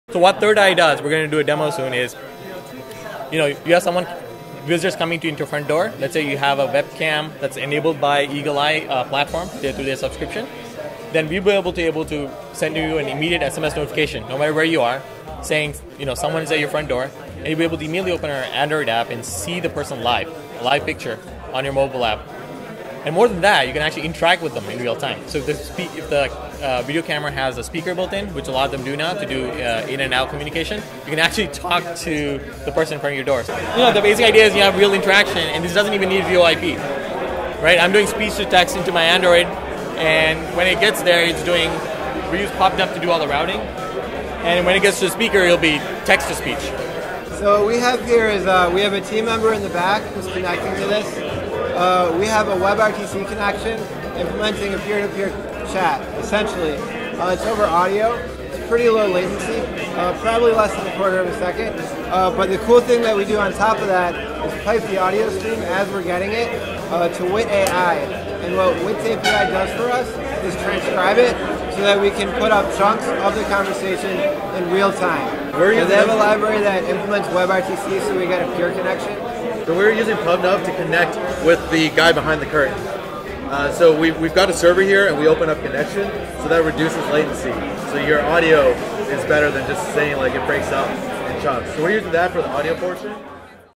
APIcon PubNub Hack Demos
Endurance was the name of the game at last week's ProgrammableWeb APIcon Hackathon. Hackers had 18 hours to build an app using the many APIs available to them, including PubNub, Evernote, Wit.AI, and Eagle Eye.

In this video, two teams that used PubNub demo their apps. 3rd Eye won 1st prize at the competition with their security alert system for connected homes and offices.